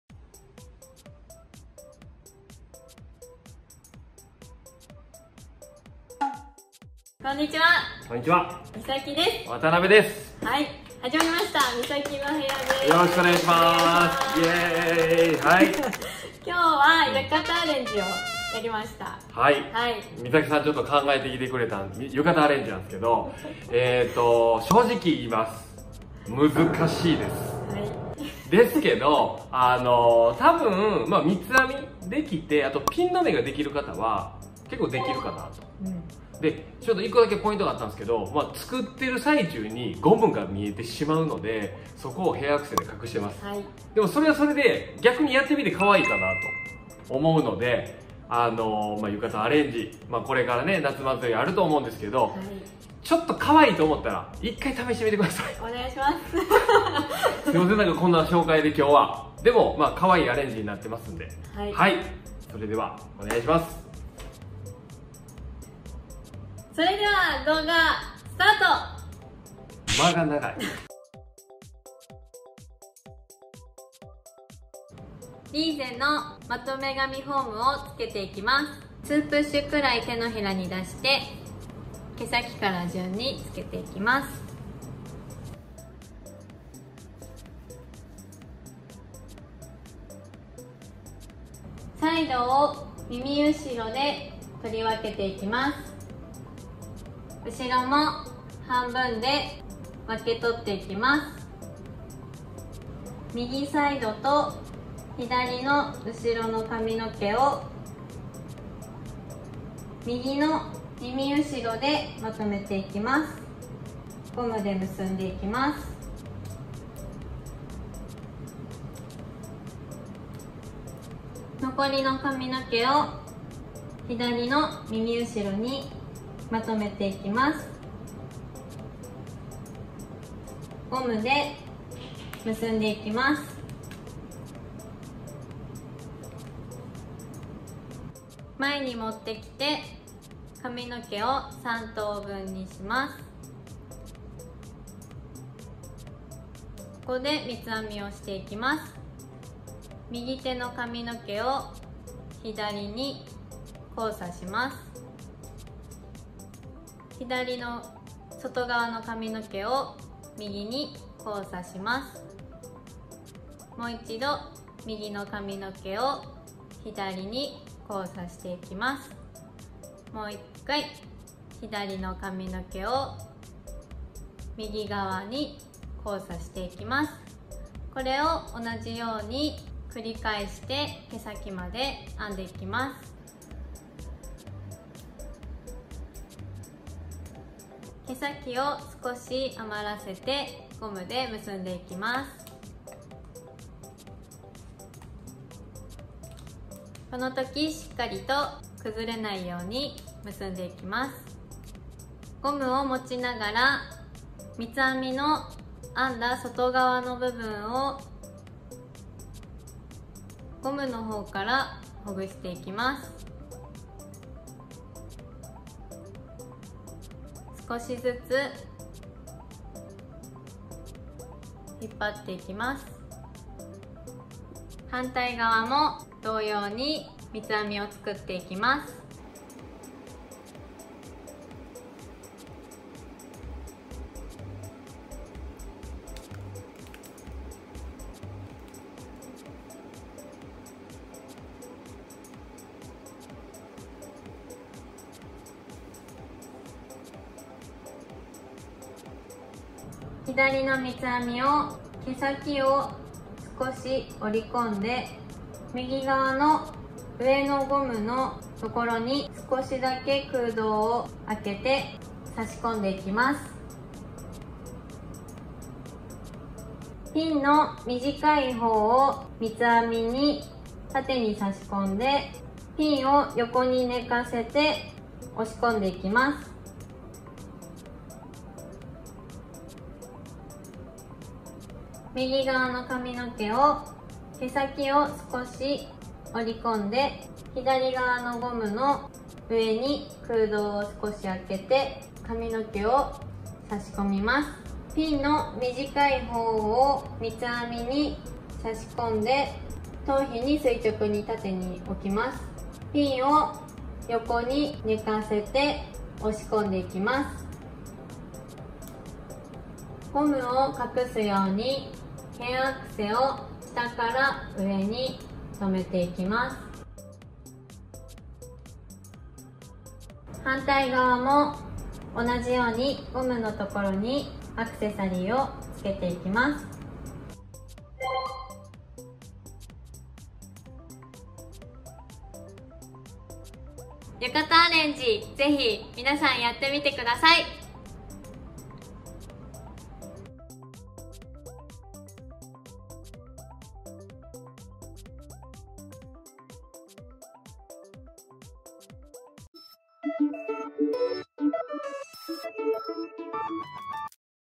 （お詫び有り）令和の浴衣アレンジは、低めの位置でまとめるのが旬！！Liese 花王 MISAKI SALONTube 渡邊義明
令和の浴衣アレンジは、
低めの位置でまとめるのが旬でオススメです！！

今回作ってみてリアルに思った事を言っております！

【使ったもの】
花王　Lieseまとめ髪フォーム
amazon
ダッカール
amazon
Puバンド
amazon
アメピン
amazon
ヘアクリップ
amazon
ヘアフック
amazon

美容動画

Hair styling
Hair arrangement
头发
헤어